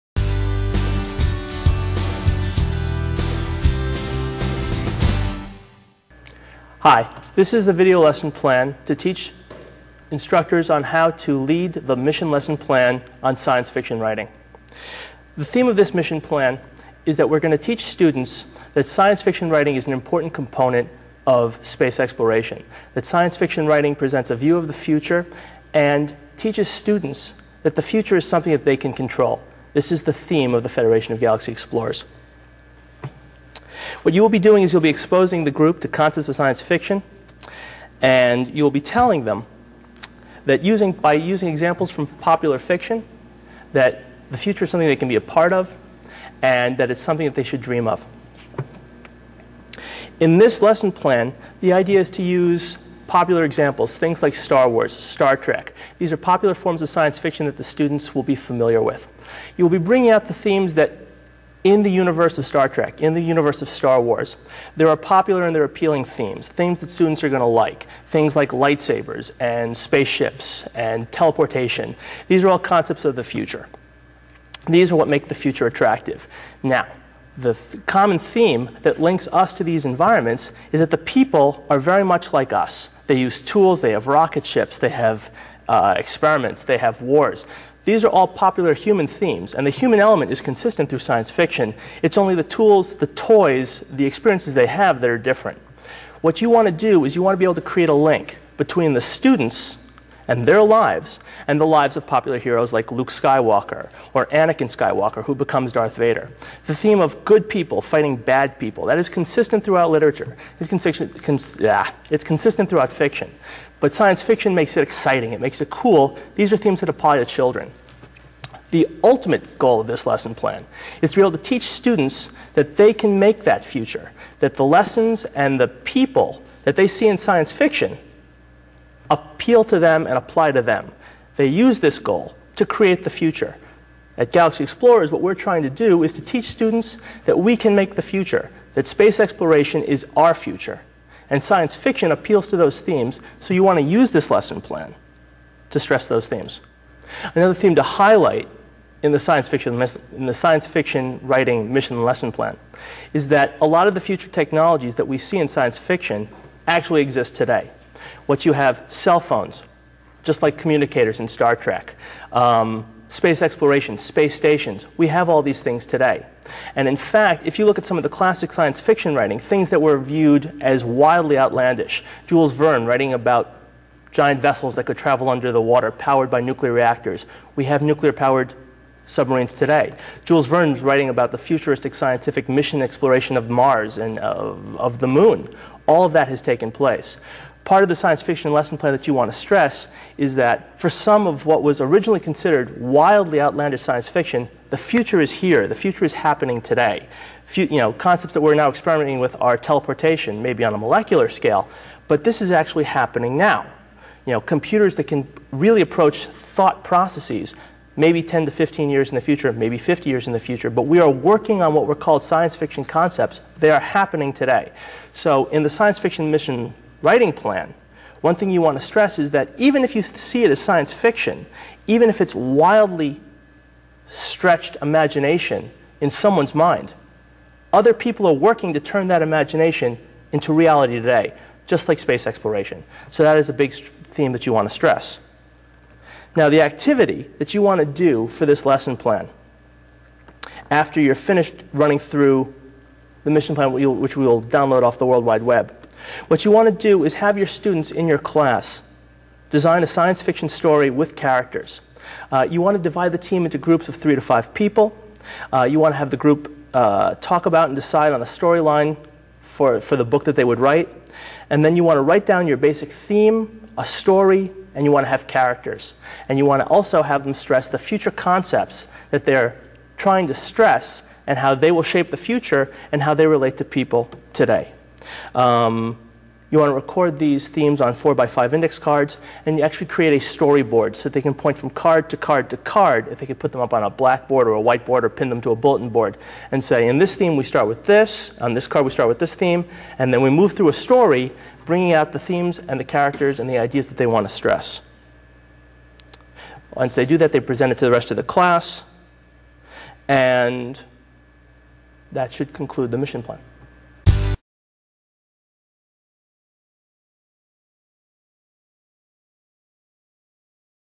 Space Science Fiction Writing Training Video
This video teaches Mission Team Leaders how to teach the Space Science Fiction Writing lesson plan